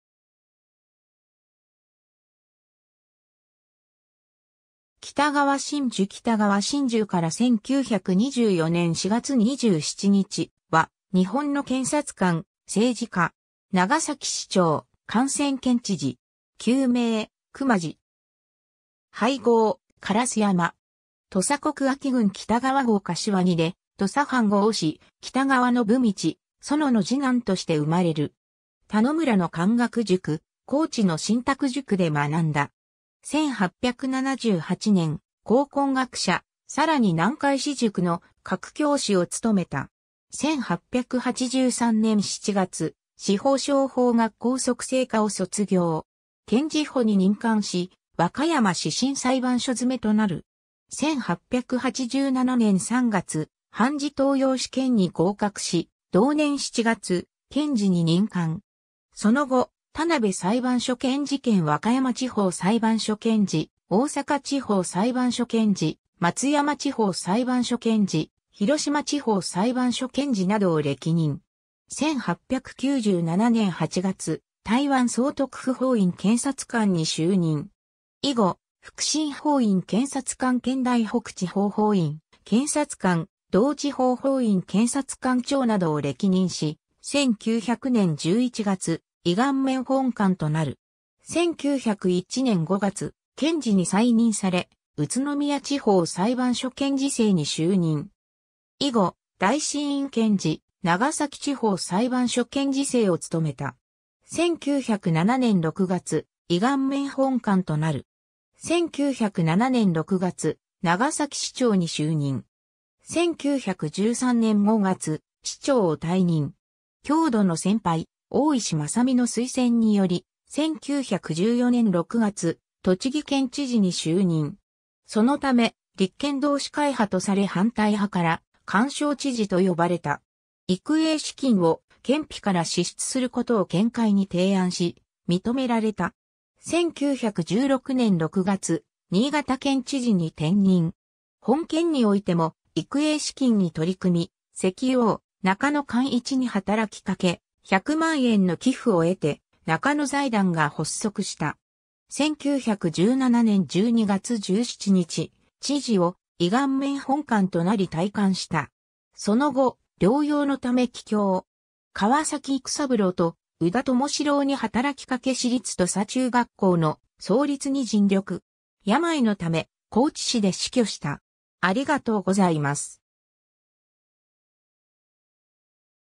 北川信従
北川信従 北川 信従（きたがわ のぶより / のぶつぐ、1860年8月1日（万延元年6月15日） - 1924年（大正13年）4月27日）は、日本の検察官、政治家。
長崎市長、官選県知事。
旧名・熊弥。
俳号・烏山。
 土佐国安芸郡北川郷柏木（現高知県安芸郡北川村）で、土佐藩郷士・北川信通、園の二男として生まれる。
田野村の漢学塾、高知の森沢塾で学んだ。
1878年、咬根学舎、さらに南海私塾の各教師を務めた。
 1883年7月、司法省法学校速成科を卒業。
検事補に任官し和歌山始審裁判所詰となる。
1887年3月、判事登用試験に合格し、同年7月、検事に任官。
その後、田辺裁判所検事兼和歌山地方裁判所検事、大阪地方裁判所検事、松山地方裁判所検事、広島地方裁判所検事などを歴任。
1897年8月、台湾総督府法院検察官に就任。
以後、覆審法院検察官兼台北地方法院検察官、同地方法院検察官長などを歴任し、1900年11月、依願免本官となる。
1901年5月、検事に再任され宇都宮地方裁判所検事正に就任。
以後、大審院検事、長崎地方裁判所検事正を務めた。
1907年6月、依願免本官となる。
 1907年6月、長崎市長に就任。
1913年5月、市長を退任。
郷土の先輩・大石正巳の推薦により、1914年6月、栃木県知事に就任。
そのため立憲同志会派とされ反対派から「干渉知事」と呼ばれた。
育英資金を県費から支出することを県会に提案し認められた。
1916年6月、新潟県知事に転任。
本県においても育英資金に取り組み、石油王・中野貫一に働きかけ、百万円の寄付を得て中野財団が発足した。
1917年12月17日、知事を依願免本官となり退官した。
 その後、療養のため帰郷。
川崎幾三郎と宇田友四郎に働きかけ私立土佐中学校の創立に尽力。
病のため高知市で死去した。